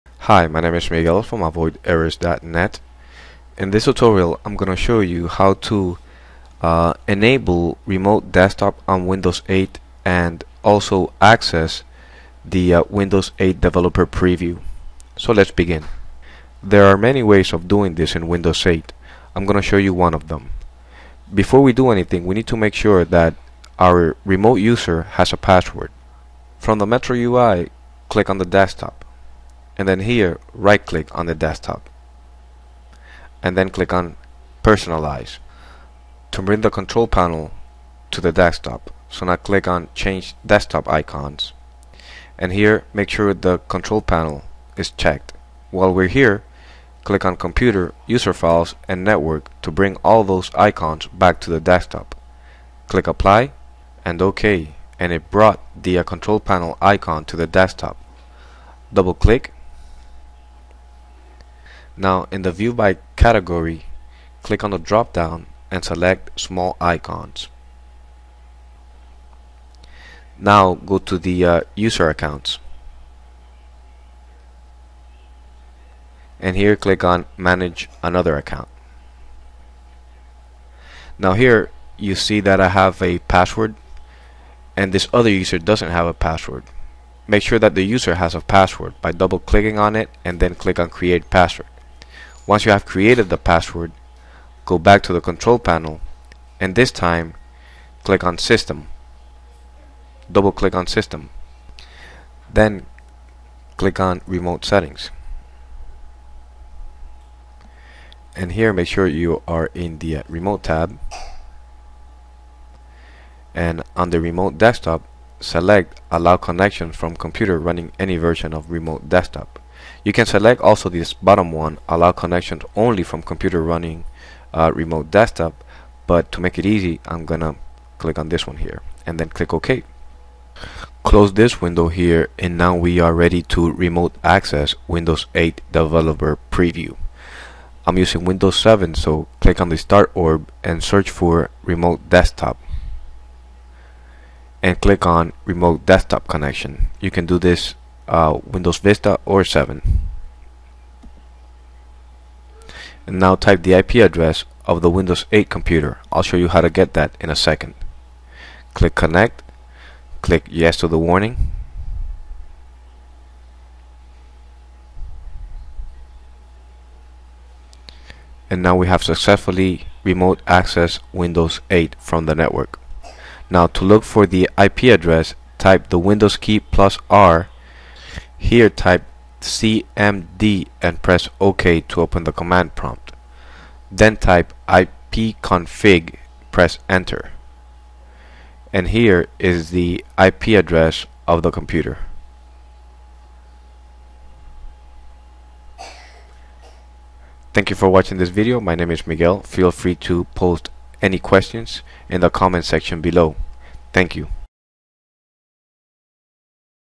How to Enable Remote Desktop on Windows 8
In this video tutorial I will show you how to enable remote desktop in Windows 8.